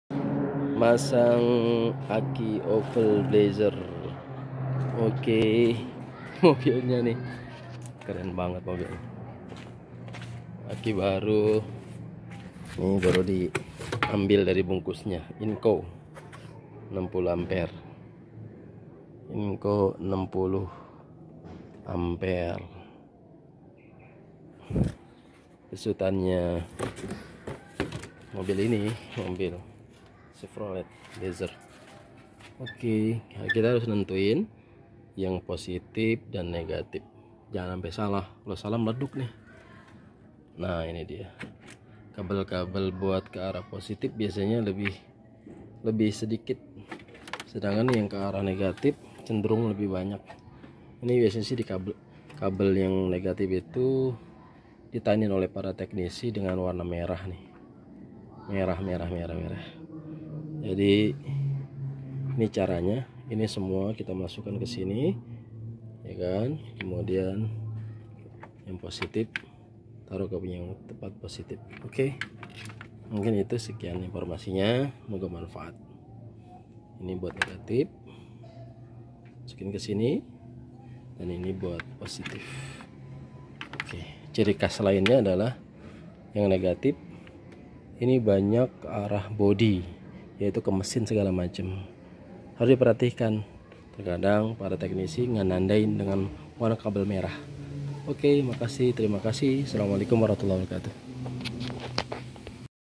SiRaja sedang dipasang energi baru'.. blazerian Indonesia
Blazer yg sudah jadi monster' blazer ini tangguh dan savety..mobil..kelas eropa emang ajiiib.yuk dahulukan keamanan. Untuk irit lebih irit dari inova' ini 2200cc loh.'jadi bandingin dengan cc yg sama ya...he2.enak dipake nanjak.Masya Allah.berkah..aamiin